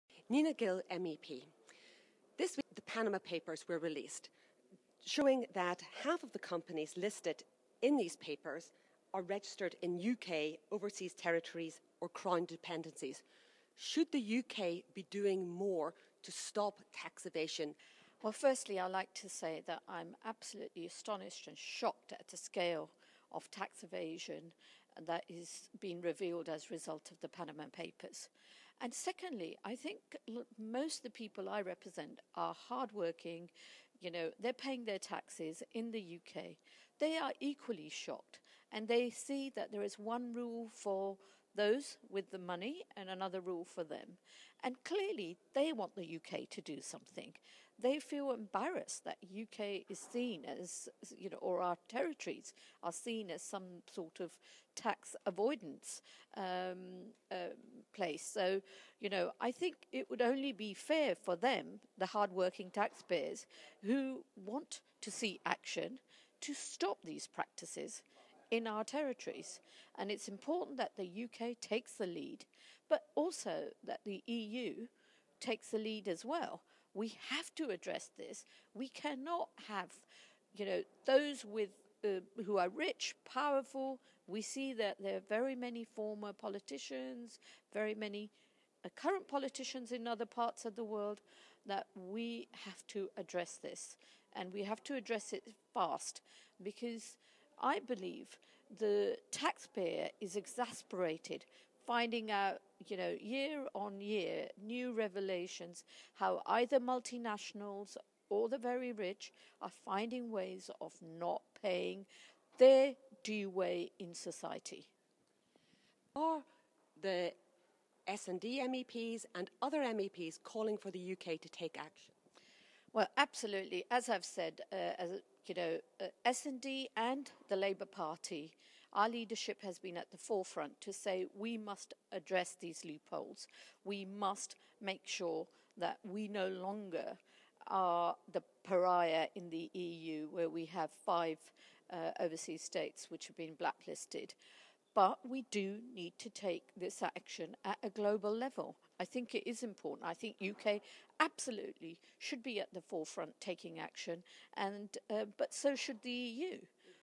PanamaPapers: Neena Gill MEP considers UK's response
More than half of the companies listed in the Panama Papers are registered in UK overseas territories or crown dependencies. Five of these also appear on the EU’s tax haven  blacklist. We asked Neena Gill MEP,  member of the European Parliament's Economic and Monetary Affairs Committee, if more could be done by the UK.